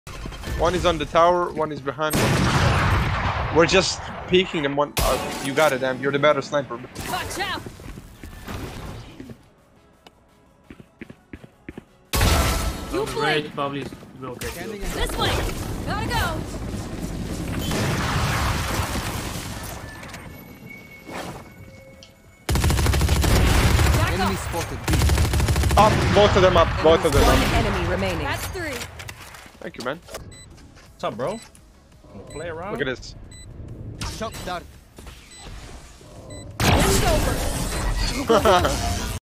"Marshall Doing What It Does Best; Annoying Operators"
What you shouldn't do POV:
Ego peeking/Overheating

Hahah but it worked this time tho XD